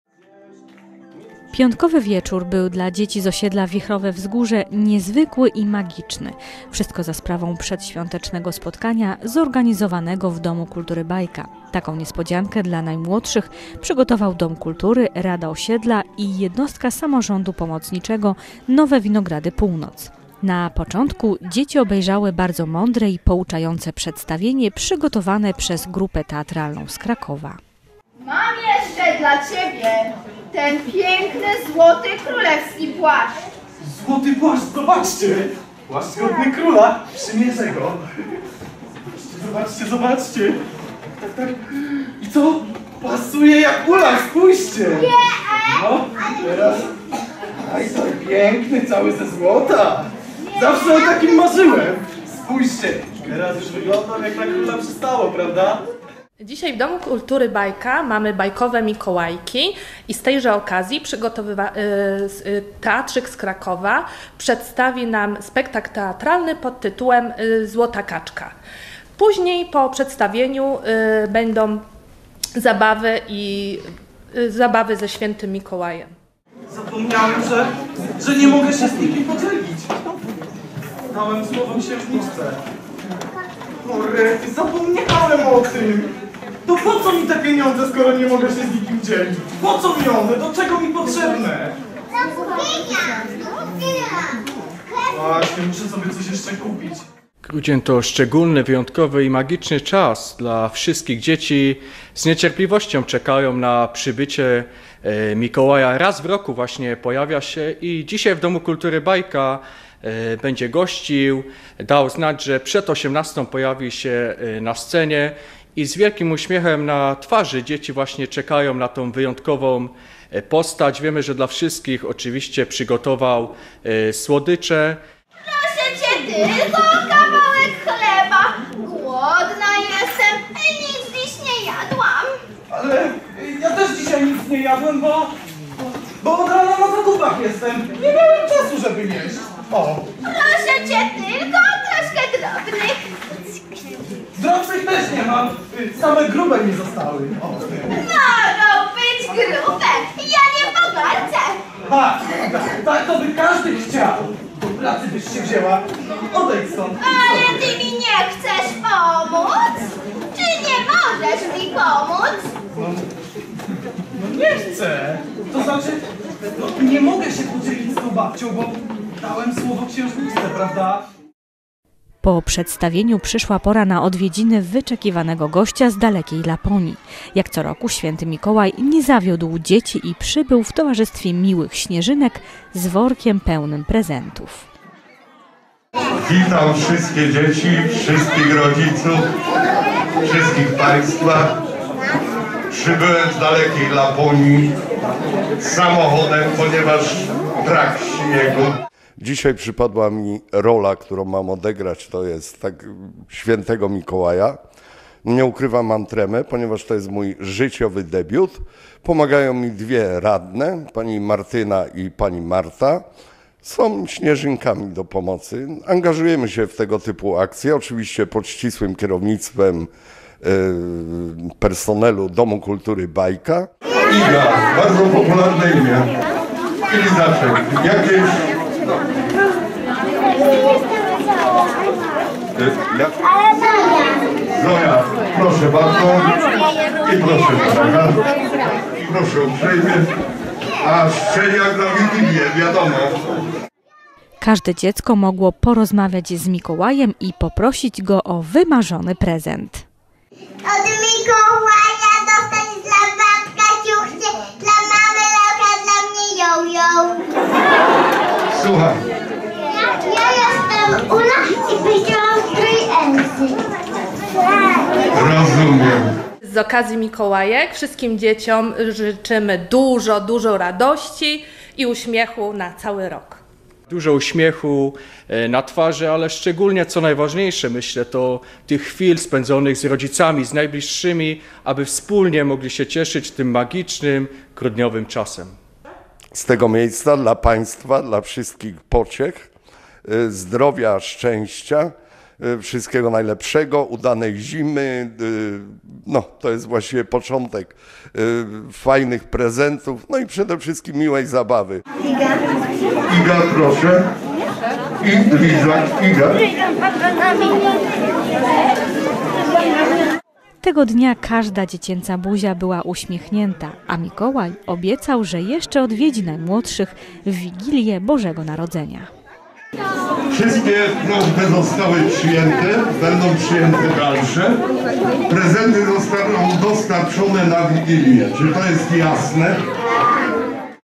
2016.12.02 Mikołajki w ODK „Bajka”
W piątek 2 grudnia w Osiedlowym Domu Kultury „Bajka” na Wichrowym Wzgórzu odbyły się BAJKOWE MIKOŁAJKI.

Organizatorem imprezy byli: Osiedlowy Dom Kultury „Bajka” na Wichrowym Wzgórzu i Rada Osiedla NOWE WINOGRADY PÓŁNOC – Jednostka Pomocnicza Miasta Poznania.

Wszystkim Dzieciom w Dniu Świętego Mikołaja
życzymy dużo uśmiechu i dużo słodyczy!

Materiał filmowy został zrealizowany przez Telewizję WTvK.